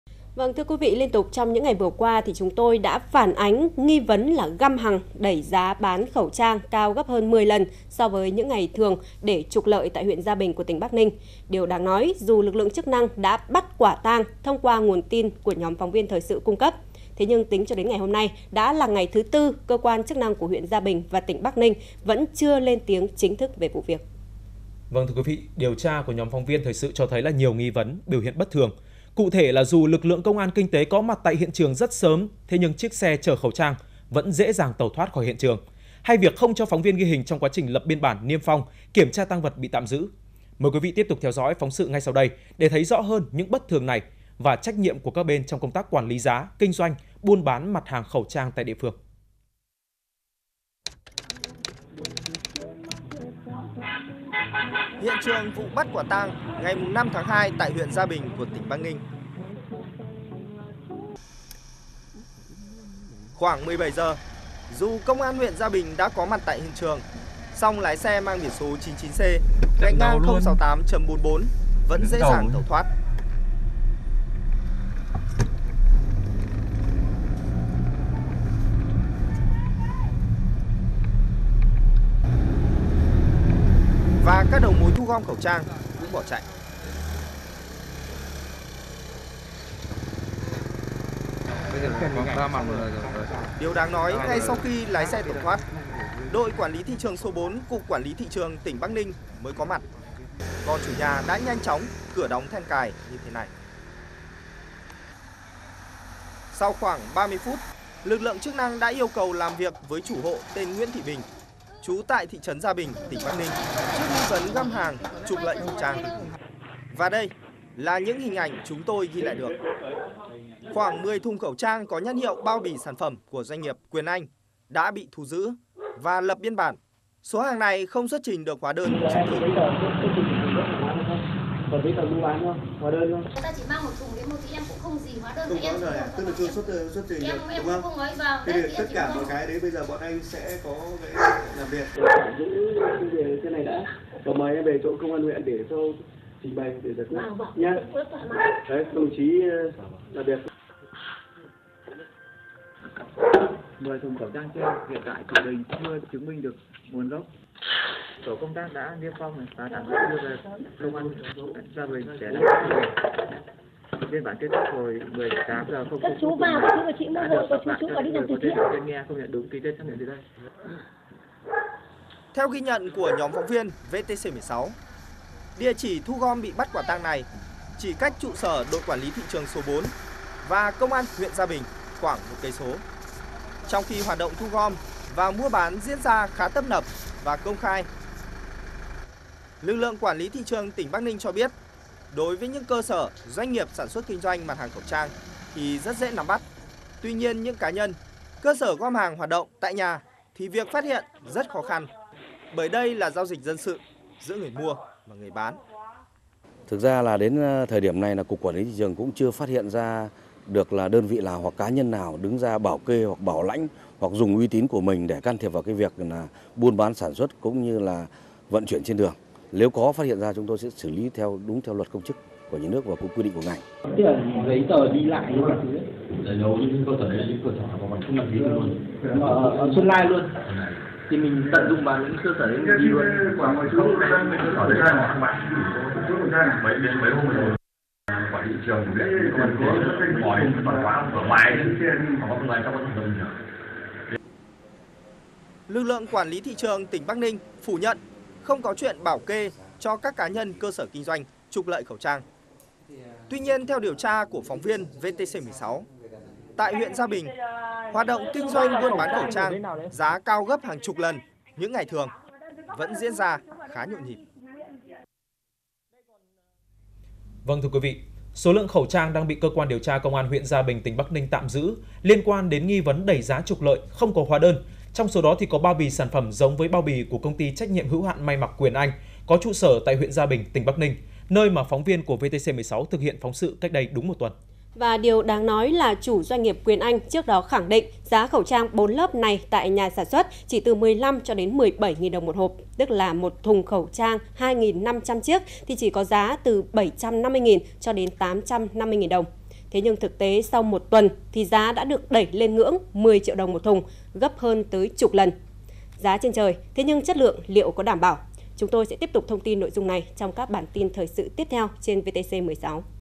Khẩu trang bị đẩy giá: Do buông lỏng quản lý? | VTC16
VTC16 | Sau nghi vấn găm hàng, đẩy giá khẩu trang cao gấp hơn 10 lần so với những ngày thường để trục lợi tại huyện Gia Bình, tỉnh Bắc Ninh. Đến nay cơ quan chức năng ở đây vẫn chưa lên tiếng chính thức về vụ việc. Có hay không việc buông lỏng quản lý?